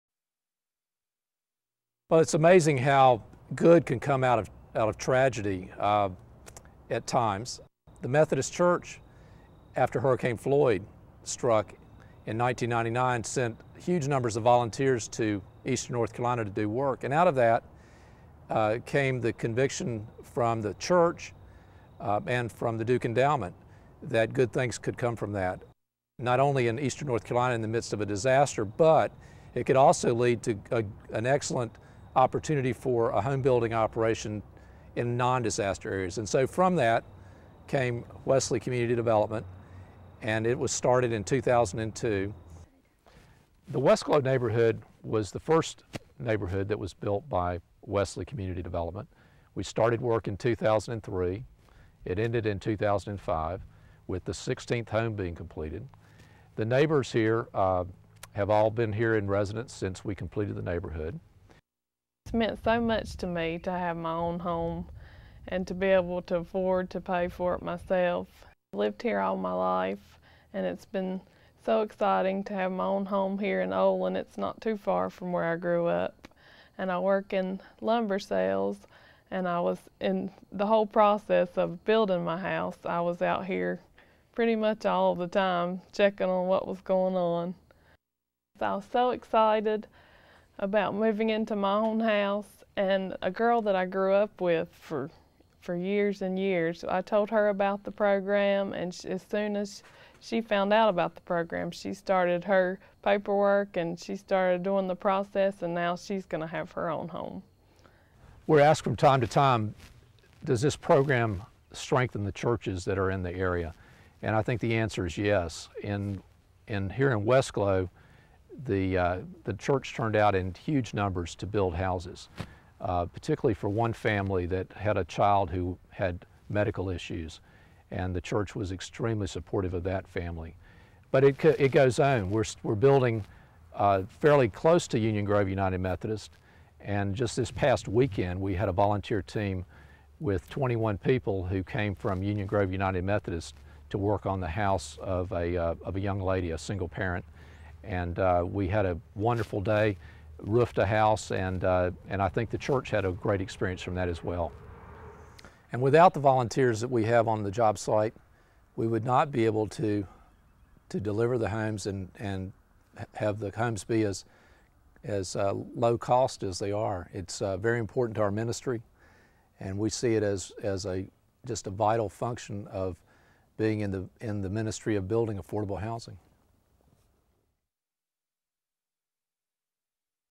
Building Affordable Homes: Wesley Community Development Corporation
Following Hurricane Floyd, The Duke Endowment established an affordable housing initiative to serve those struggling to own their own homes.